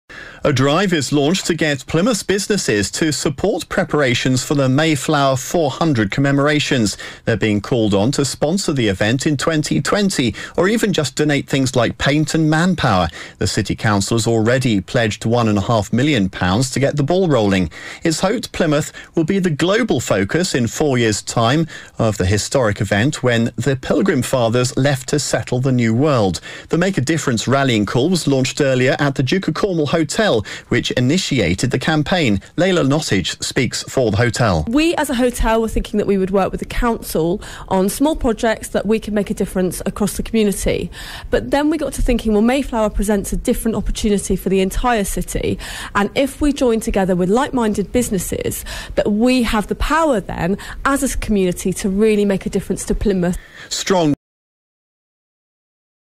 MAYFLOWER 400 - Radio Plymouth Interview
Make a Difference for Mayflower 400 - Radio Plymouth feature with an  interview from out Head of Marketing Leila Nottage.